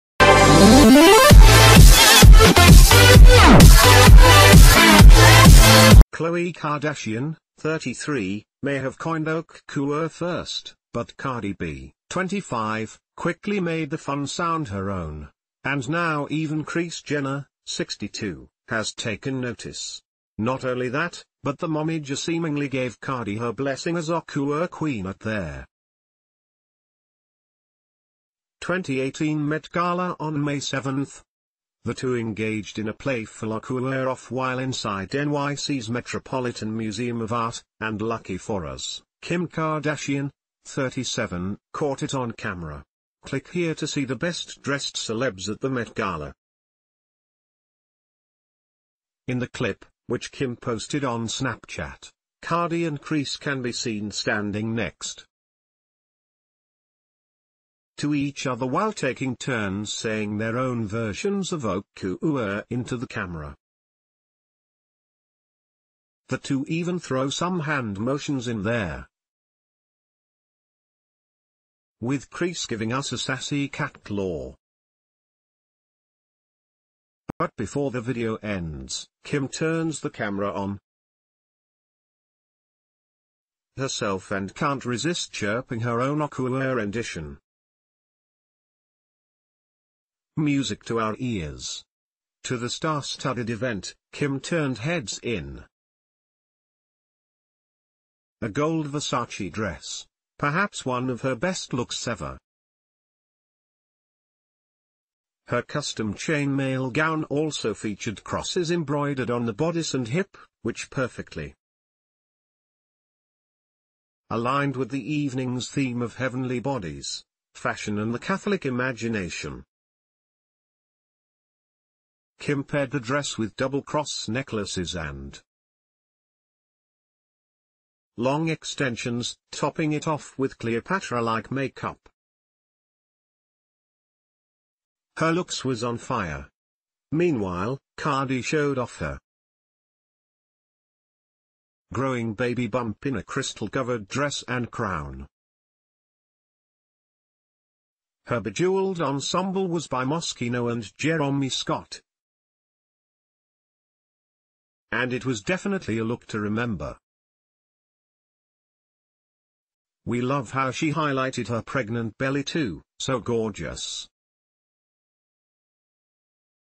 Cardi B Takes Kris Jenner & Kim Kardashian’s ‘Okuurrr’ Game To Next Level At Met Gala
Khloe Kardashian, 33, may have coined “okkkuuurrr” first, but Cardi B, 25, quickly made the fun sound her own — and now even Kris Jenner, 62, has taken notice! Not only that, but the momager seemingly...